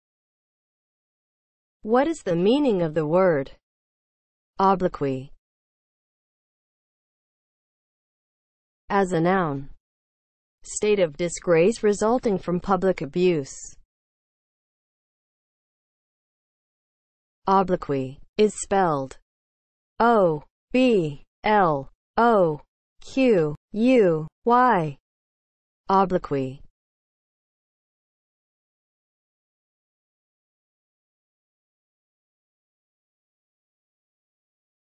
What is the meaning of the word OBLOQUY?
Definition and spelling of the word OBLOQUY.

(00:00) Intro
(00:07) As a noun
(00:15) Spelling

As a noun (00:07):
• state of disgrace resulting from public abuse